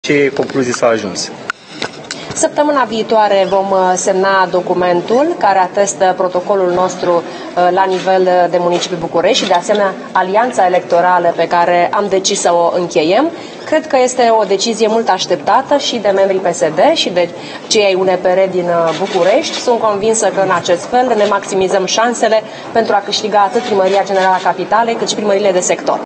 Declarații de presă după Conferința Extraordinară de Alegeri a Organizației PSD Sector 5
Gabriela Firea despre Protocolul cu UNPR
Titlul evenimentului: Declarații de presă după Conferința Extraordinară de Alegeri a Organizației PSD Sector 5